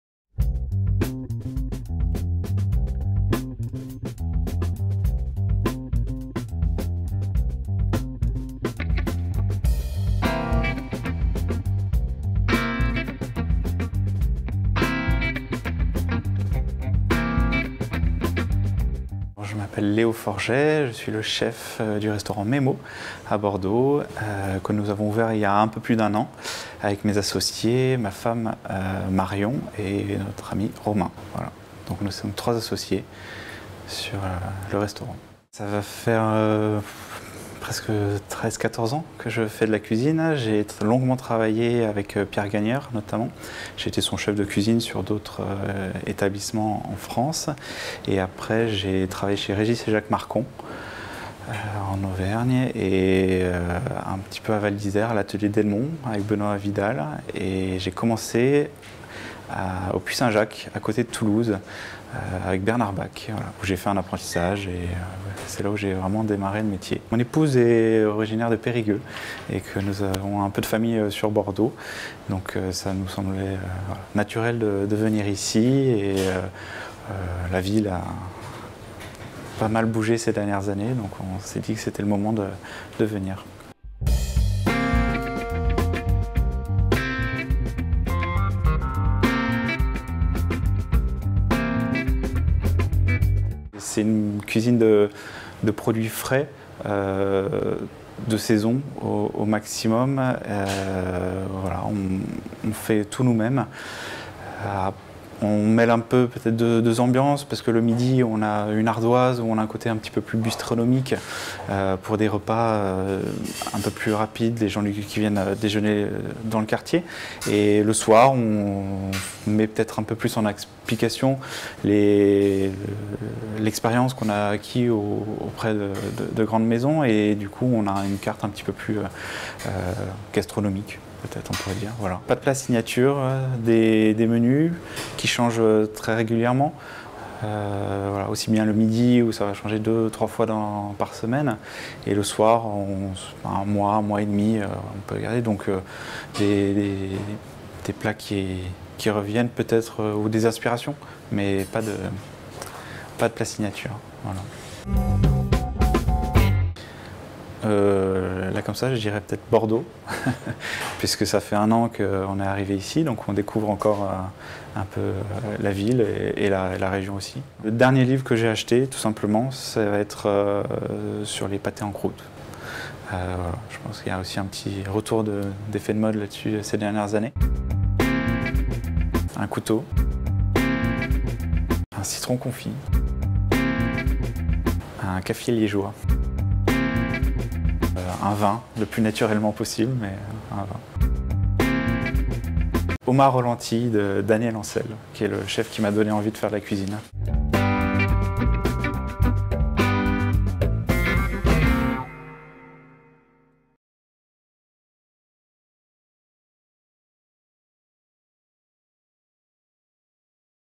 Des nouvelles des fourneaux - Mets Mots
Rencontre avec Léo Forget, talentueux chef du restaurant bordelais "Mets Mots".
Ouvert il y a un peu plus d'un an, Mets-Mots est un lieu chaleureux où les clients peuvent déguster des produits frais et de saison avec une ardoise bistronomique le midi et une carte davantage gastronomique le soir.